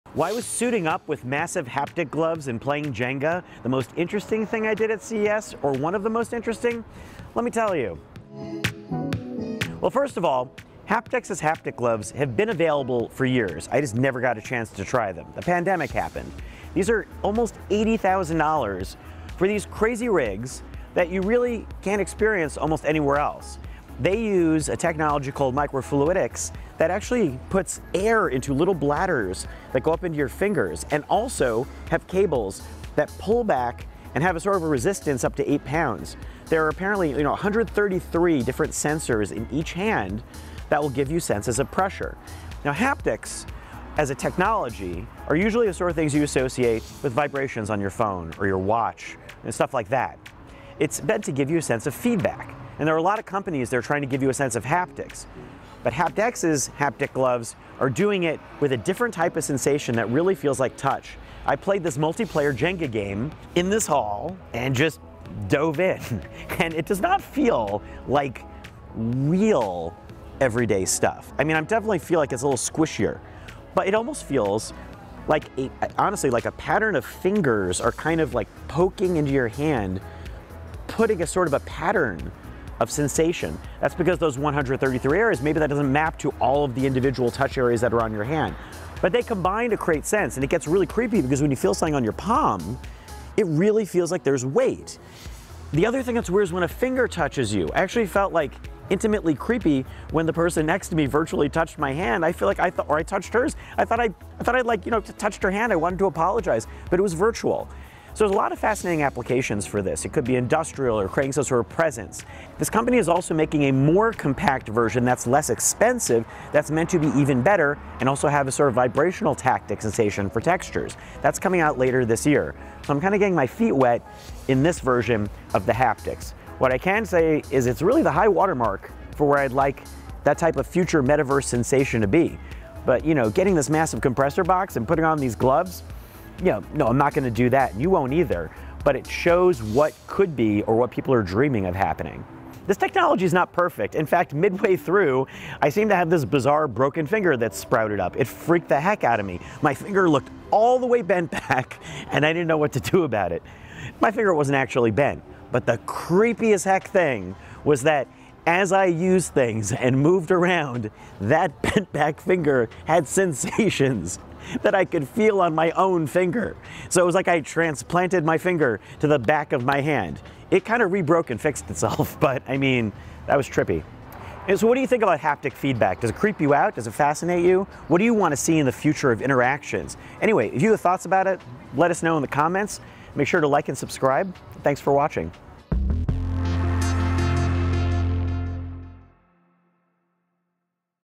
Jenga With HaptX Gloves Threw My Hands Into Virtual Reality
With a new model coming soon, we caught up with the best haptic gloves out there to see what multiplayer board games felt like.